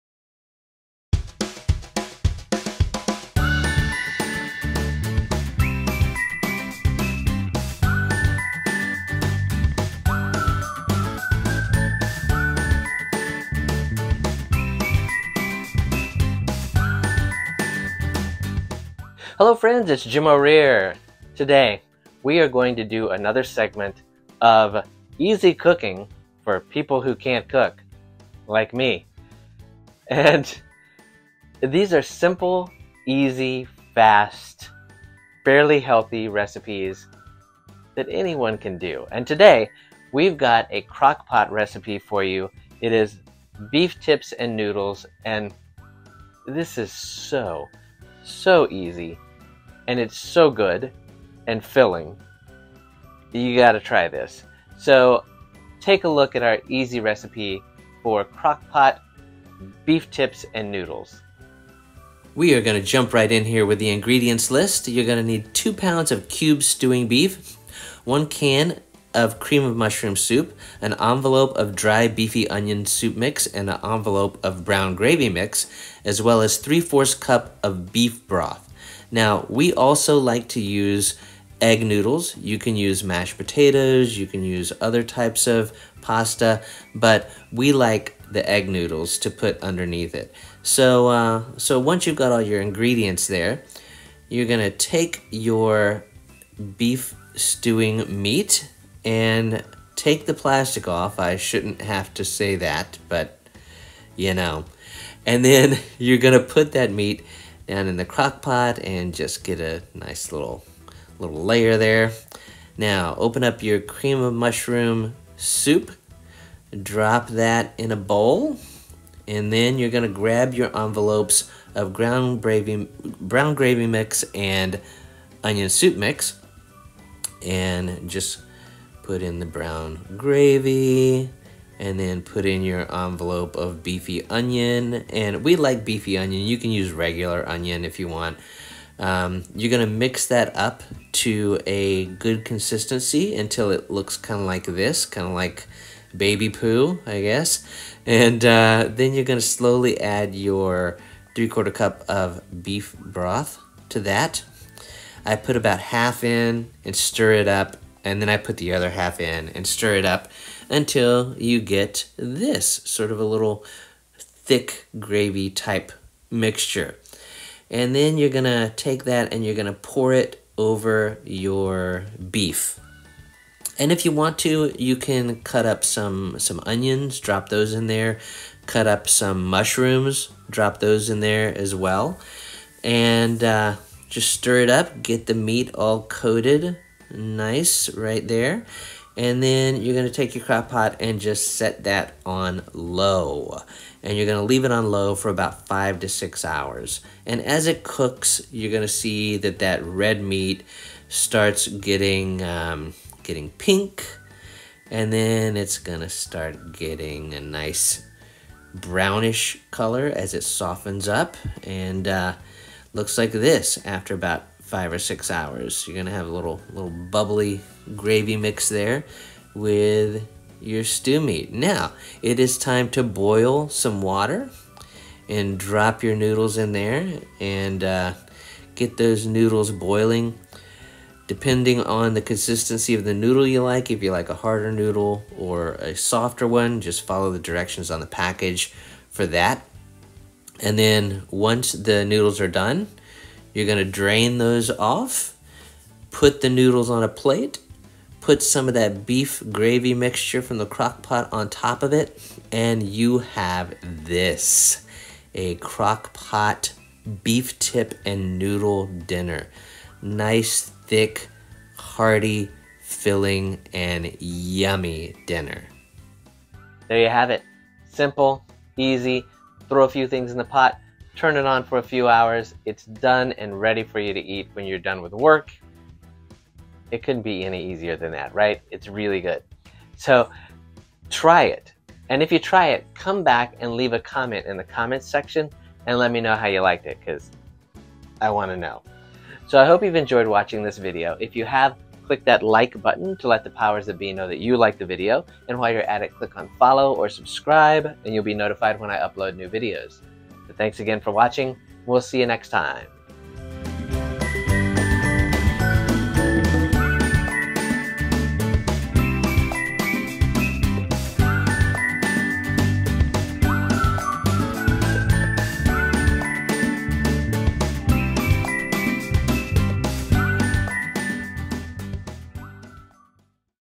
Deliciously Simple Crock Pot Beef Tips & Noodles Recipe!
Discover the ultimate comfort food with our mouthwatering Crock Pot Beef Tips & Noodles Recipe! Easy to make, packed with flavor, and perfect for any occasion. Learn how to create tender beef tips and savory noodles with our step-by-step guide. Impress your family and friends with this deliciously simple dish that will leave everyone asking for seconds! Watch now and satisfy your cravings with every hearty bite!

CHAPTERS:
00:00 Introduction
01:01 Ingredients
01:33 Preparing The Meat
01:53 Preparing The Topping
03:12 Cooking The Meat
03:42 Preparing The Noodles
04:11 The Finished Product
04:26 Recap